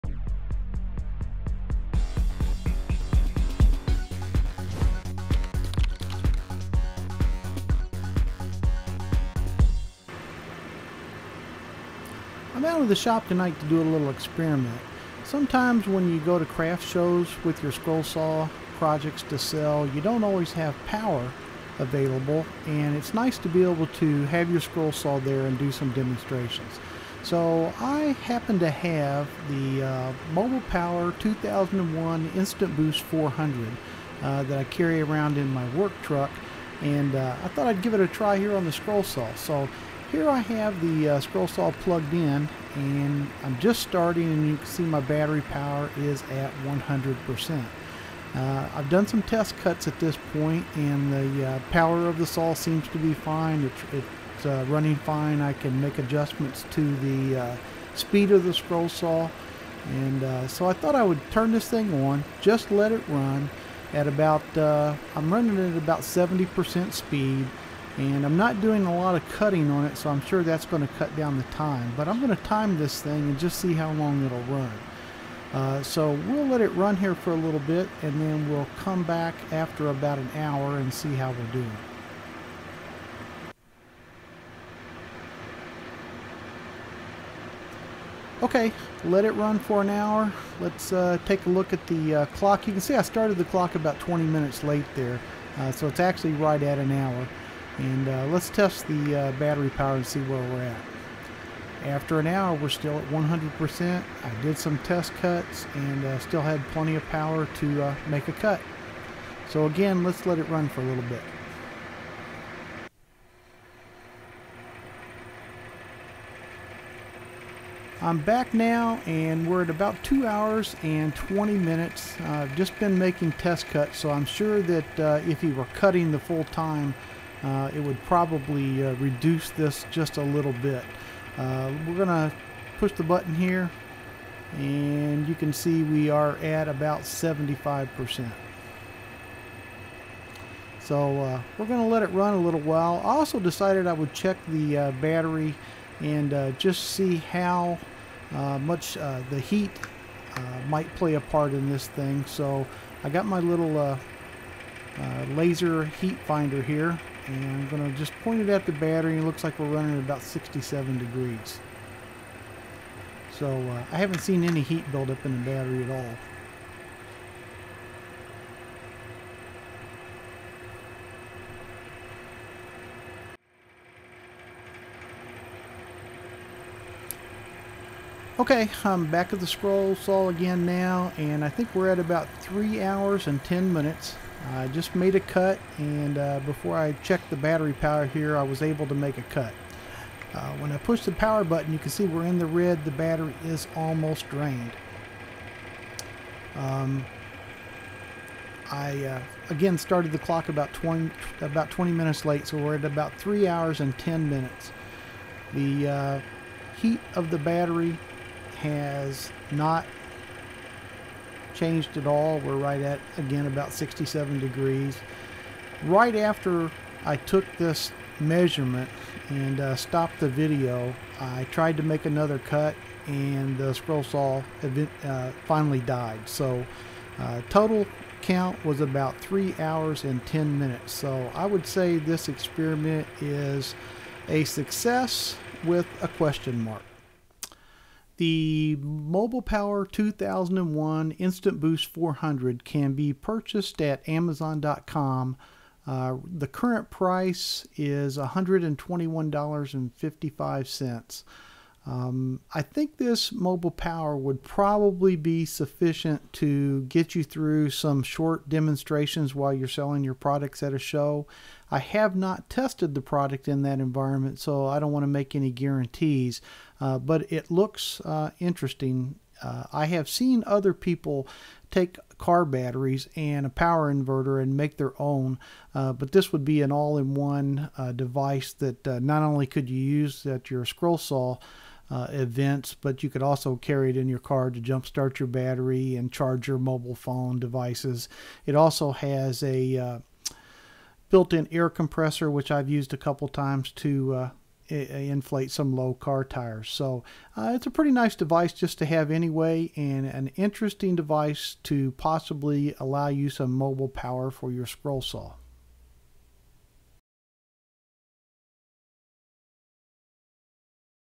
Portable power for the scroll saw. Great for demonstrations.
Posted 7/6/2015 on the blog. More info on the blog.